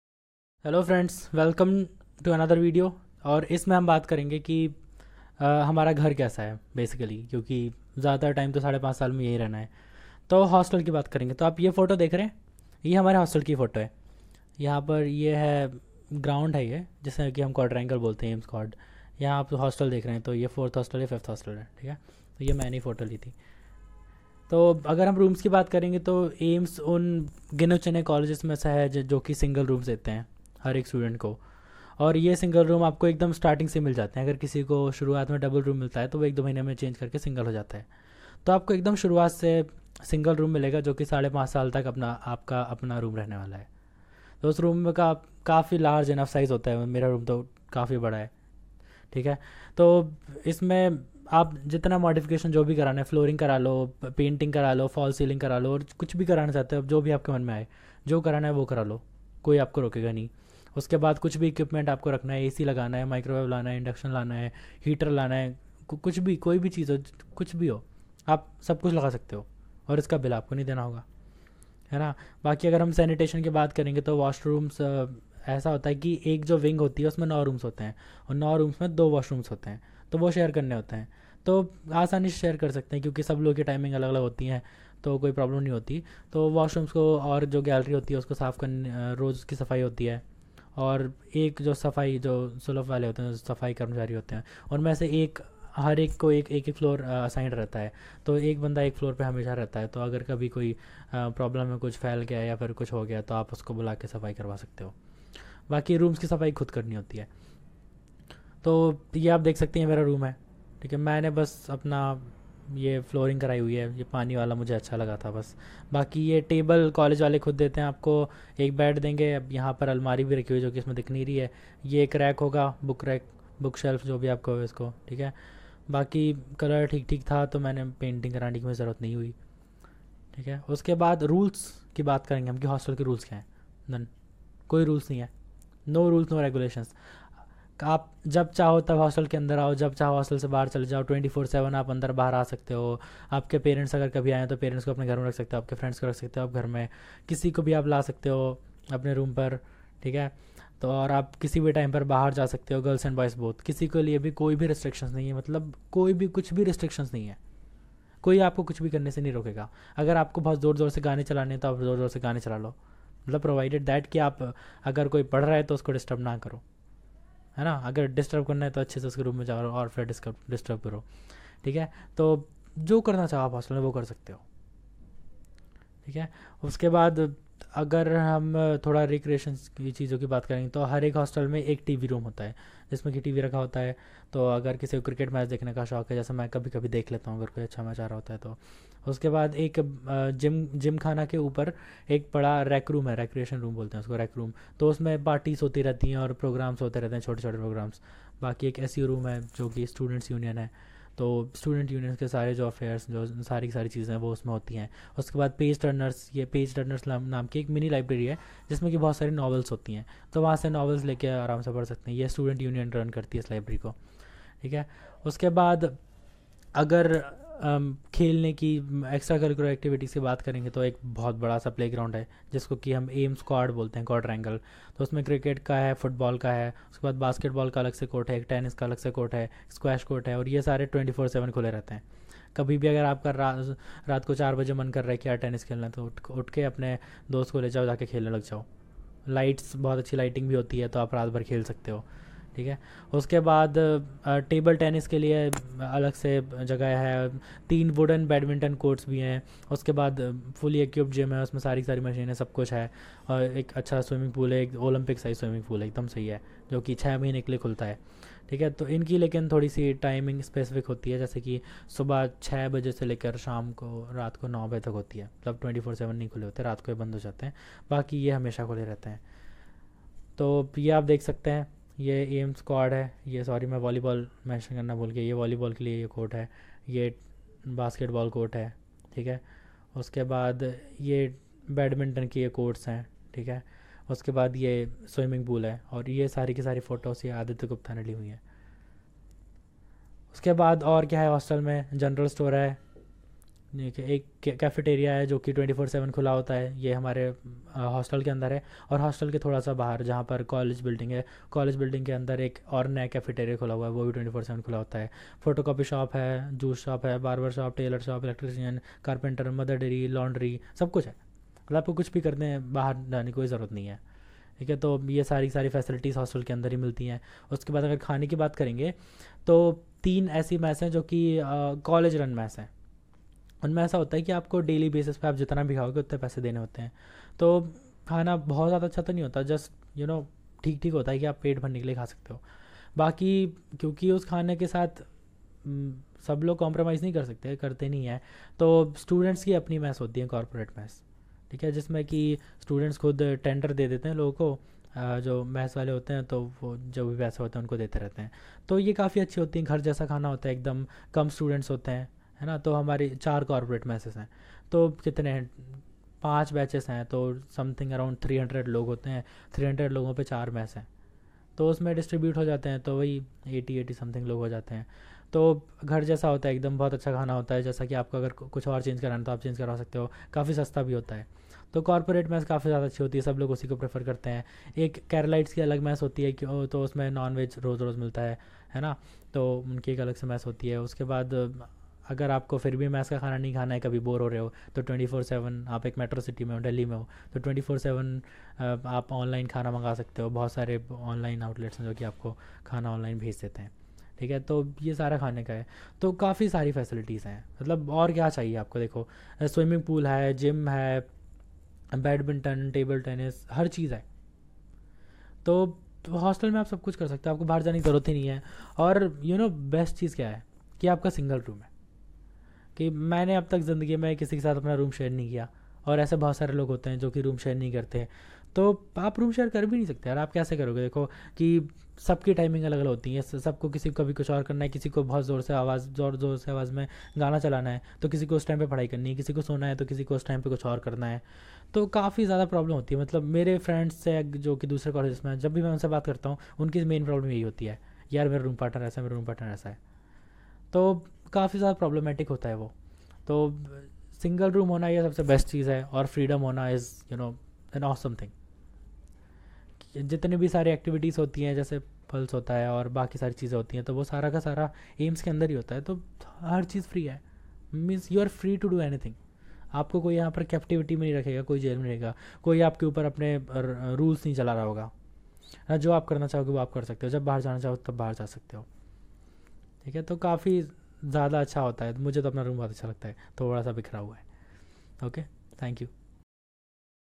AIIMS Delhi - Hostel Life - कैसी है AIIMS के होस्टल की ज़िन्दगी
In this video, Hariom Patel takes you through the hostel life of AIIMS in Delhi. If you are an aspirant of AIIMS 2018, then it will be very interesting for you to know about the student life in AIIMS. Especially if it is coming from an existing student who lives on the coveted campus. Know about AIIMS Delhi Campus, get inspired and work hard to crack the entrance exam .